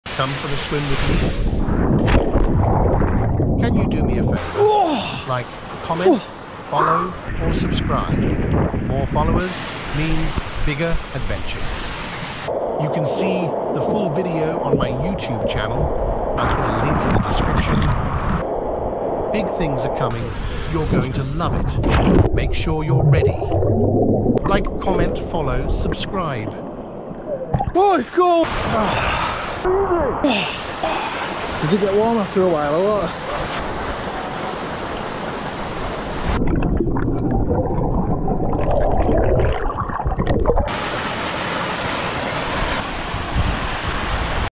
Come swim with me at a Hidden Gem along the Watkins Path.
Short video showing me jumping off the Rocks and Swimming in the Infinity Pools along the Watkins Path

this is after climbing Snowdon.

full video is on my channel